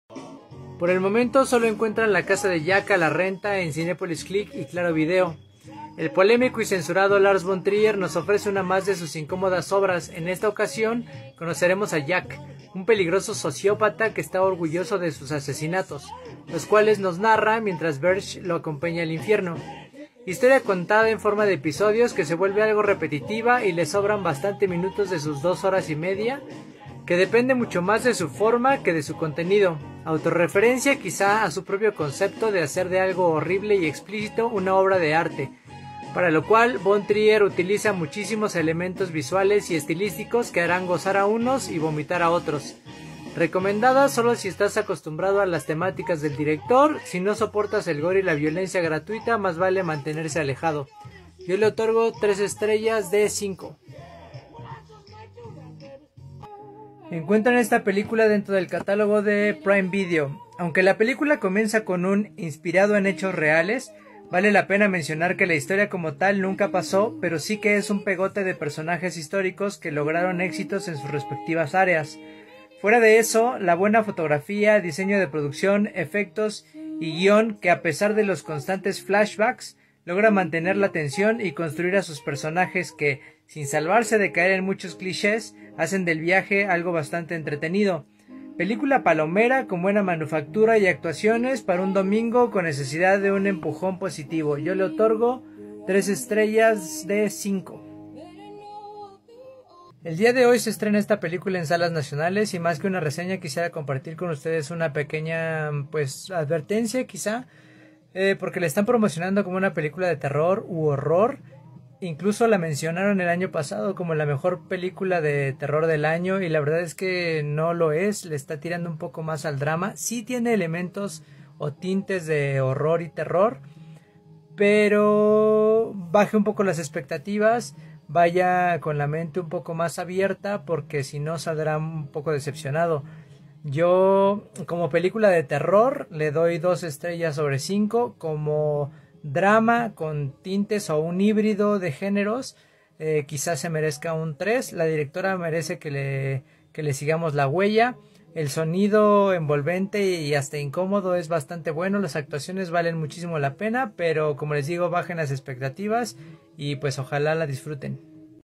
Reseña Express de "La casa de Jack", "Aeronautas" y "Relic"
Reseña Express de:
"La casa de Jack" (2018) que encuentras en MUBI
"Aeronautas" (2019) que encuentras en Amazon Prime
"Relic"  (2020) que encuentras en Star+